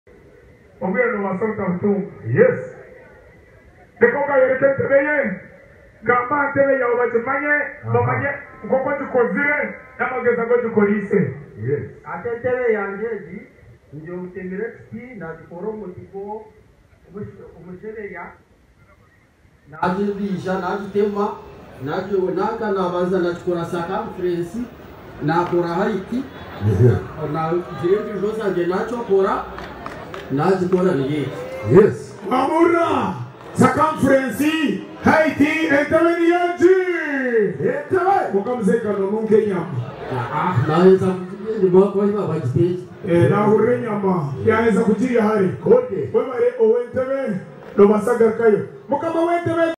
THE MAN MADE A STOOL FOR MUHOOZI KAINERUGABA WANTED HIM TO SIT ON IT
PROMOTER BRONSON
RUSSIA VS UKRAINE